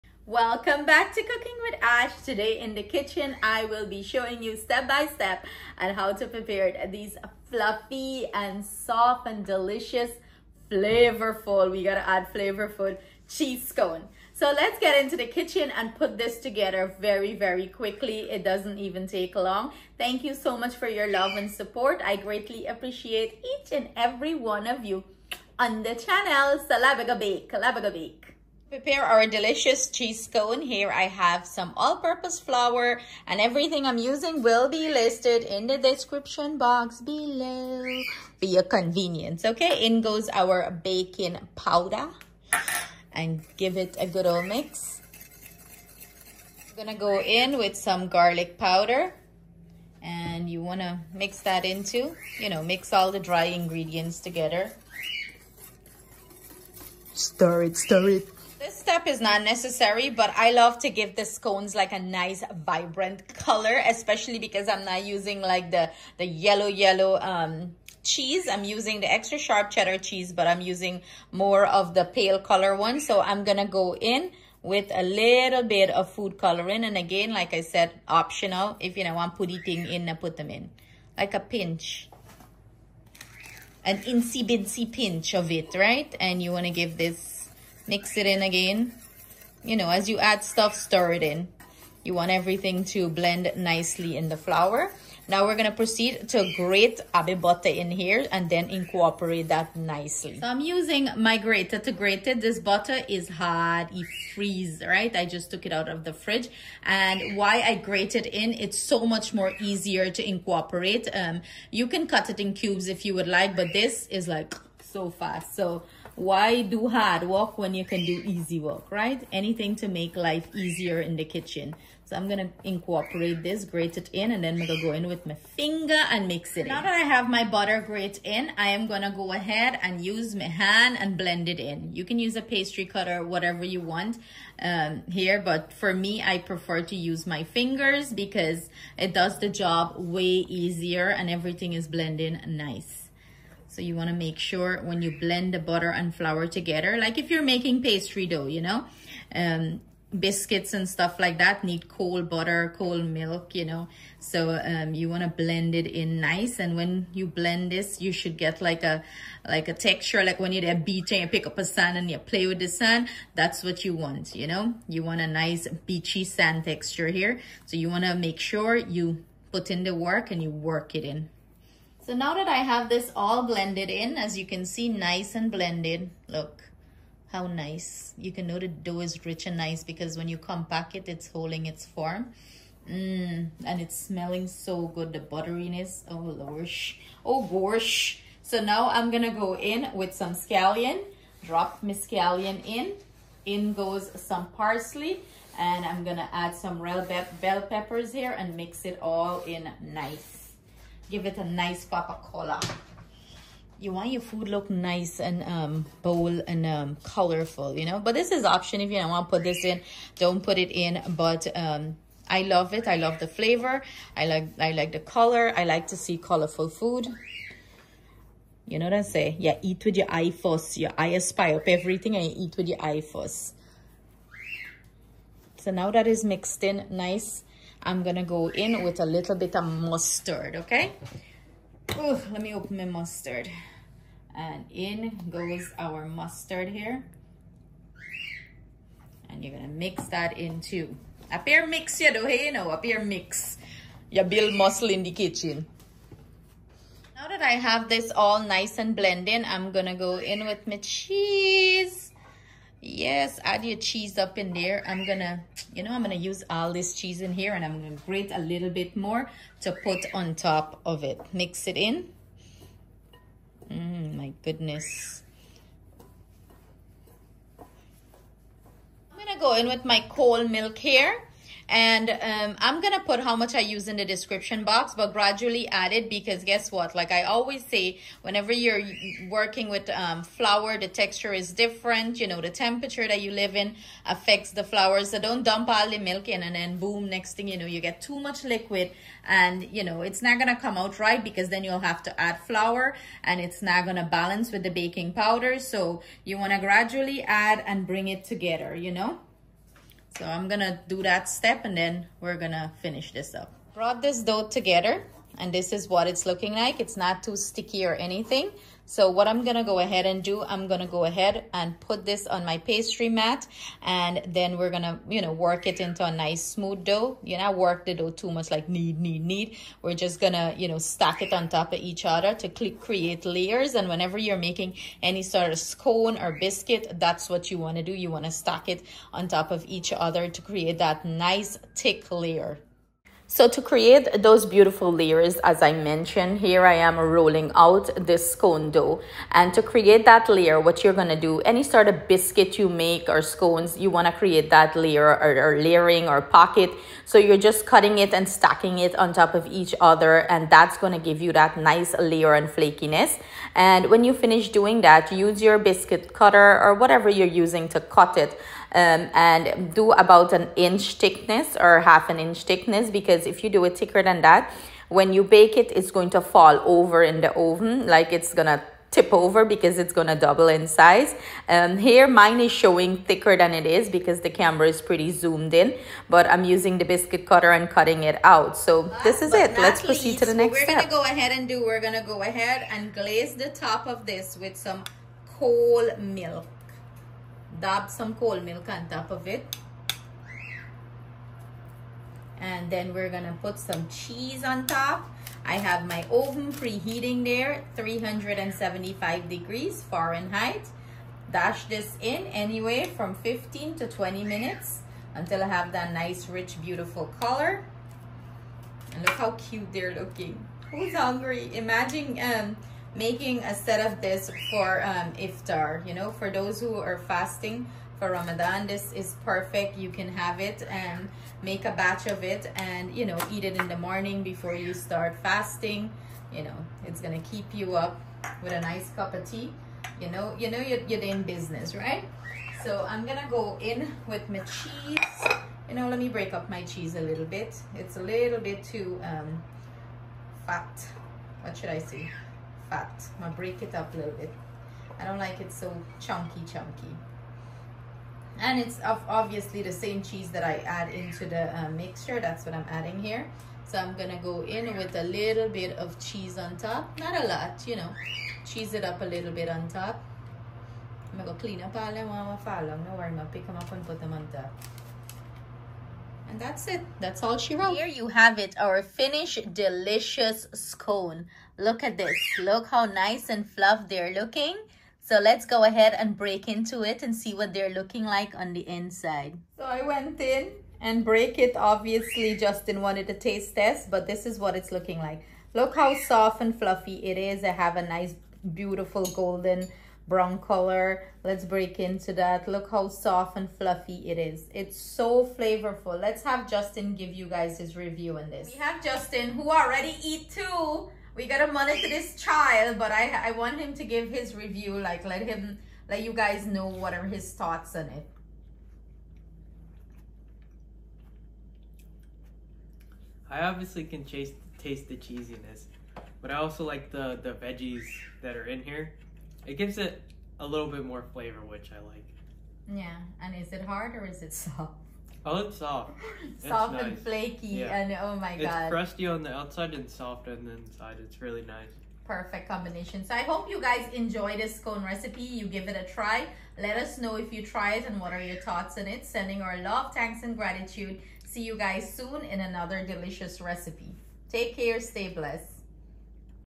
Delicious Cheese Scone Rich & Flavorful
Ingredients
2 1/4 cup flour
3 1/2 tsp baking powder
1 1/2 cup sharp cheddar cheese
6 tbsp butter
1 cup cold milk
1 tsp mustard
Pinch of yellow food coloring
1 small bell pepper
2 scallion
Parsley
1/2 tsp garlic powder

Topping
1/3 cup whole milk
1/2 cup cheddar cheese

Bake 375 for 15 minutes